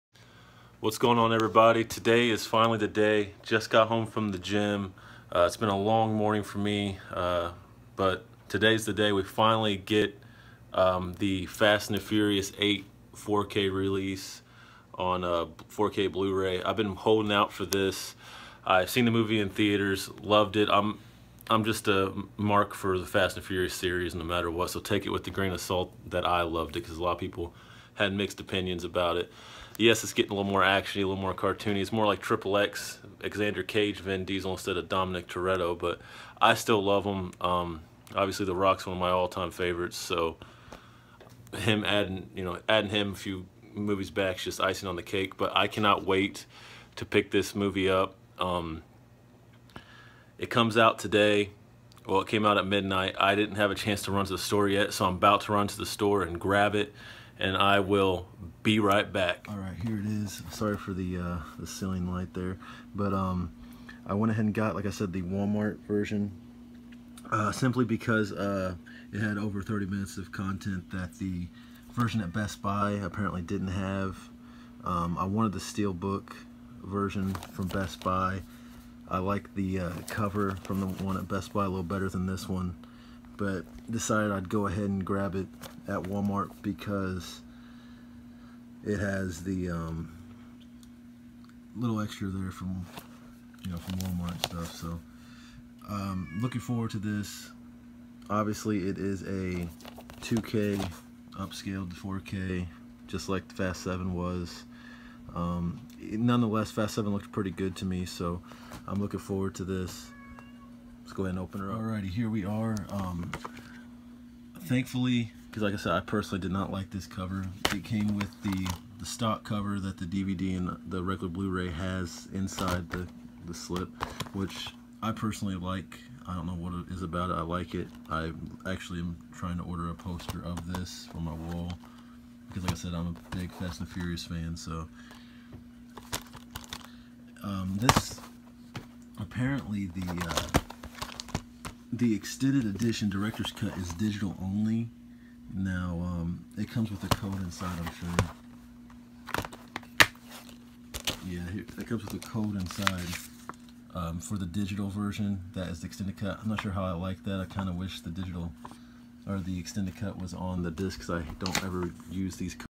Fate Of The Furious 4k Limited Collector Edition Review
This is me opening the new Walmart exclusive 4k blu ray release of fast8
The Fate of the Furious
In the eighth installment of the Fast and the Furious series, Dominic Toretto's crew are rocked to their core when they must face their most dangerous adversary yet: Dom himself (Vin Diesel), who is now working with a shadowy cyber-terrorist known only as Cipher (Charlize Theron). As DSS agent Luke Hobbs (Dwayne Johnson) and the rest of the team attempt to fight b...